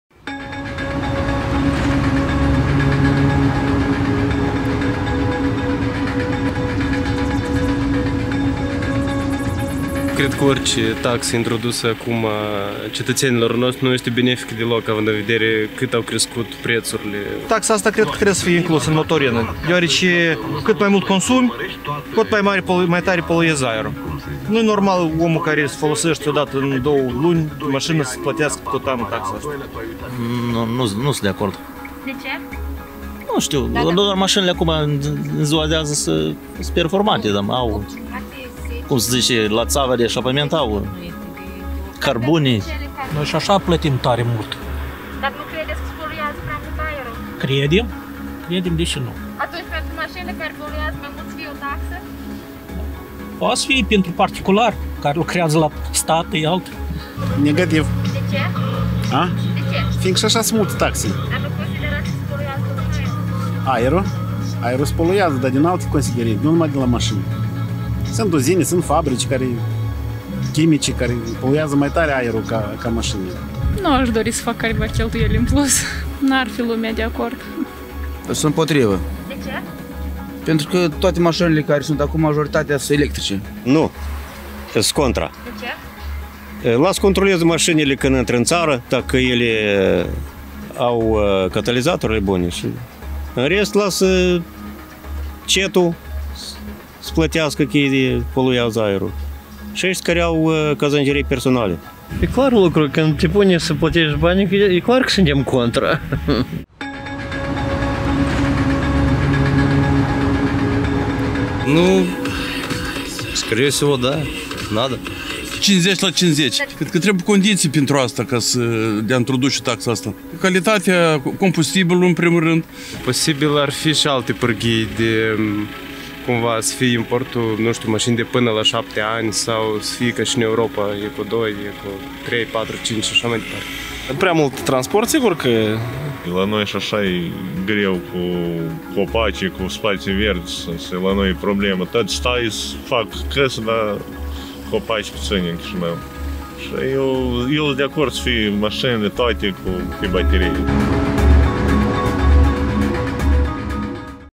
Pro sau Contra? Ce spun soferii de la Chisinau despre o eventuala taxa de mediu
Principala sursă de poluare a aerului în Republica Moldova, în special în municipiul Chișinău, sunt mașinile, având o pondere de peste 90% din totalul emisiilor de poluanți. Pentru a reduce nivelul de poluare, Ministerul Mediului a propus în cadrul exercițiului de elaborare a politicii bugetar-fiscale și vamale pentru anul 2022, introducerea taxei de mediu în prețul carburanților, iar mijloacele financiare ce ar urma să fie acumulate la bugetul de stat să fie utilizate pentru finanțarea programelor și proiectelor de împădurire și gestionare a fondului silvic. Totuși, propunerea nu a mai ajuns să devină lege. Cum am ajuns în anul 2022 fără o taxă de poluare pentru carburanți, care este viziunea autorităților pe acest subiect, ce cred experții, dar și cum taxa de mediu reduce din apetitul pentru mașinile de lux în alte state.

👀 Pe INSTAGRAM vezi ce mai facem zi de zi: @agoramoldova